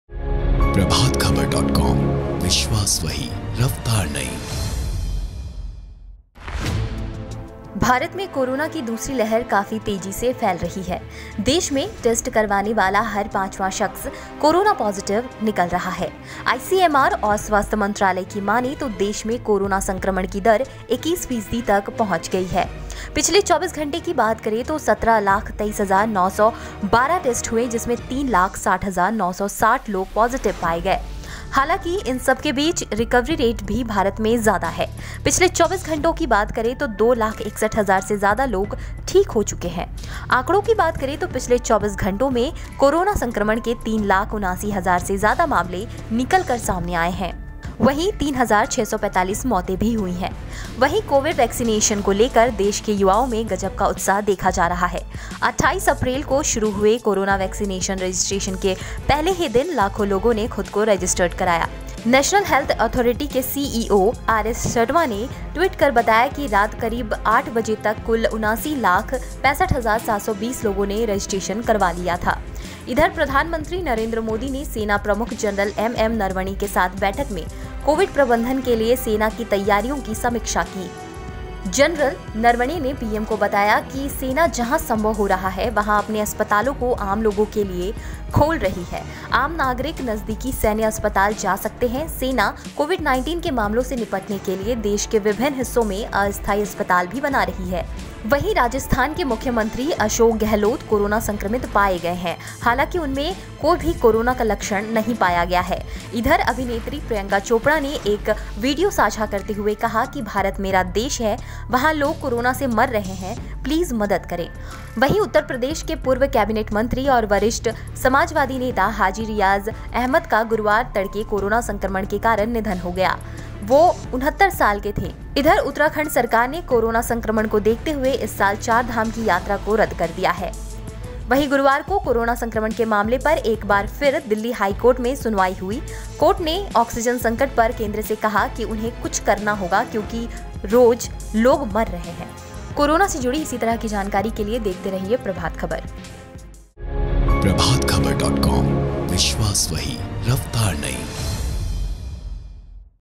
भारत में कोरोना टेस्ट के बाद हर पांचवां आदमी पॉजिटिव, सेना ने कहा- जहां संभव वहां पहुंचा रहे मदद
Corona India Update: भारत में कोरोना टेस्ट के बाद हर पांचवां आदमी पॉजिटिव, सेना ने कहा- जहां संभव वहां पहुंचा रहे मदद | Prabhat Khabar
भारत में कोरोना की दूसरी लहर काफी तेजी से फैल रही है... देश में टेस्ट करवाने वाला हर पांचवां शख्स कोरोना पॉजिटिव निकल रहा है.. ICMR और स्वास्थ्य मंत्रालय की मानें तो देश में कोरोना संक्रमण की दर 21 फीसदी तक पहुंच गई है. पिछले 24 घंटे की बात करें तो 17 लाख 23 हजार 912 टेस्ट हुए जिसमें 3 लाख 60 हजार 960 लोग पॉजिटिव पाए गए.. हालांकि इन सब के बीच रिकवरी रेट भी भारत में ज्यादा है.. पिछले 24 घंटों में 2 लाख 61 हजार से ज्यादा लोग ठीक हो चुके हैं... आंकड़ों की बात करें तो पिछले 24 घंटों में कोरोना संक्रमण के 3 लाख 79 हजार से ज्यादा मामले निकल कर सामने आए हैं. वहीं, 3645 मौतें भी हुई हैं..वहीं, कोविड वैक्सीनेशन को लेकर देश के युवाओं में गजब का उत्साह देखा जा रहा है.. 28 अप्रैल को शुरू हुए कोरोना वैक्सीनेशन रजिस्ट्रेशन के पहले ही दिन लोगों ने लाखों की संख्या में खुद को रजिस्ट्रेशन कराया. नेशनल हेल्थ अथॉरिटी के सीईओ आरएस शर्मा ने ट्वीट कर बताया कि रात करीब आठ बजे तक कुल 79 लाख 65 हजार 720 लोगों ने रजिस्ट्रेशन करवाया.. इधर प्रधानमंत्री मोदी ने सेना प्रमुख जनरल एम एम नरवणे के साथ बैठक में कोविड प्रबंधन के लिए सेना की तैयारियों की समीक्षा की. जनरल नरवणे ने पीएम बताया कि सेना जहां संभव हो रहा है.